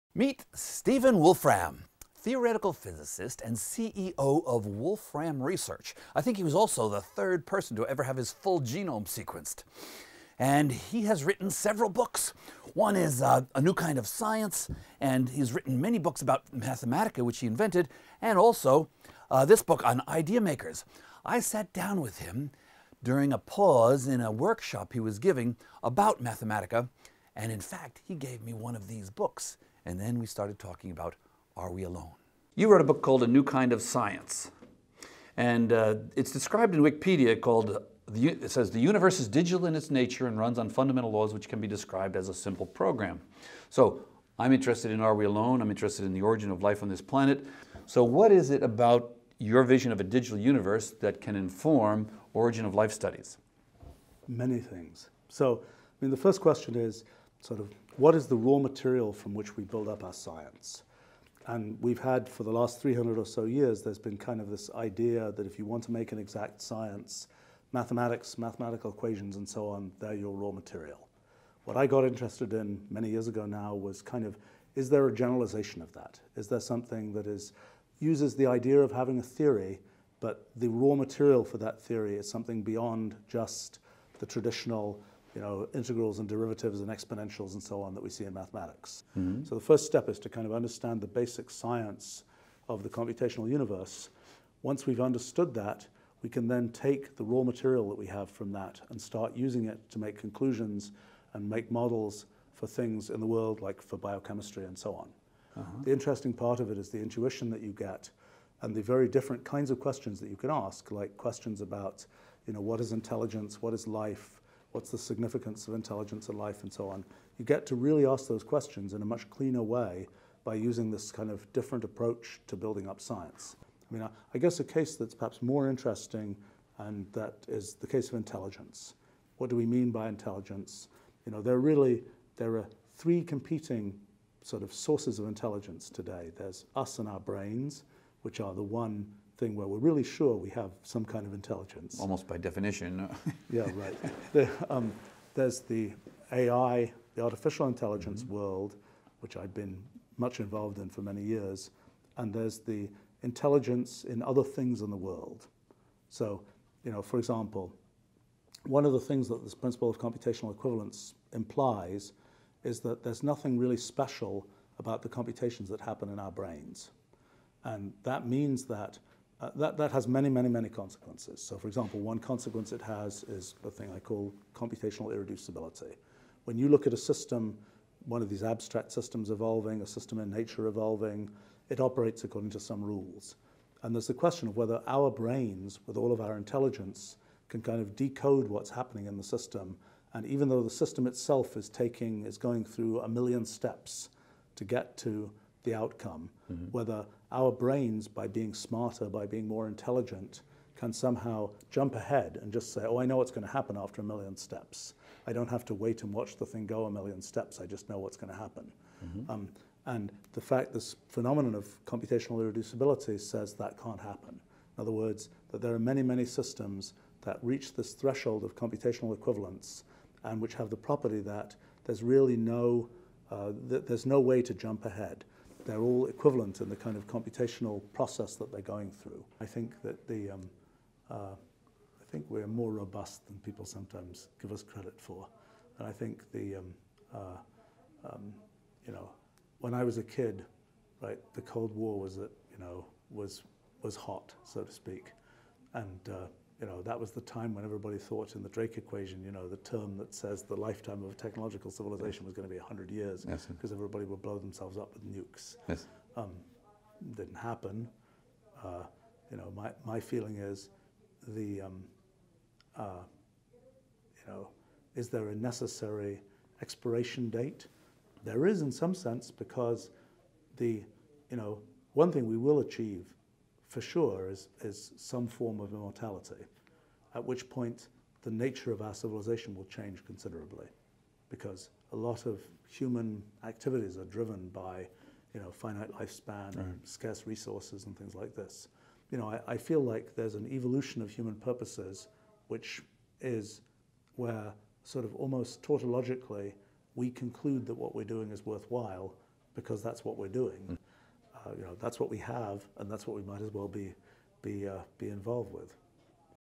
1.4.7 Stephen Wolfram: theoretical physicist/founder of Mathematica
Stephen Wolfram (theoretical physicist and inventor of Mathematica): says we can use the basic science of the computational universe to answer questions like: What is life? What is intelligence? One thing we will achieve for sure is some form of immortality at which point the nature of our civilisation will change considerably.